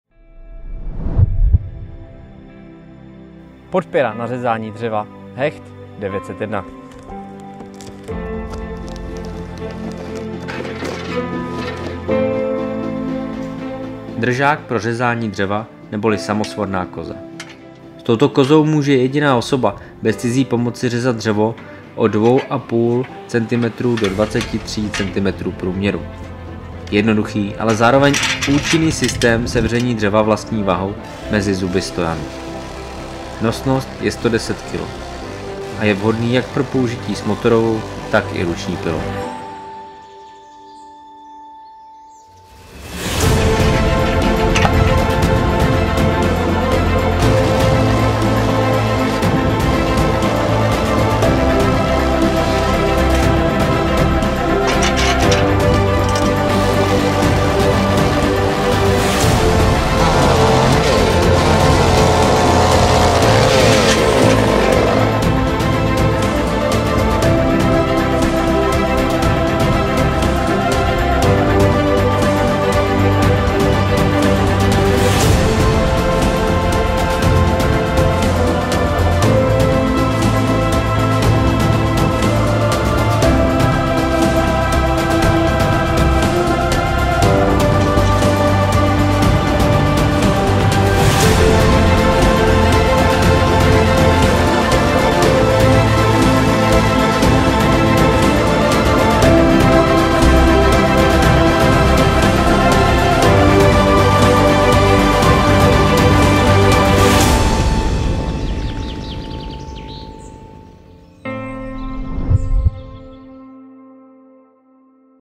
Stojan na řezání dřeva - HECHT 901
Samosvorná koza - držák pro řezání dřeva. Umožňuje řezat dřevo od 25 mm do 230 mm průměru. Nosnost je 110 kg.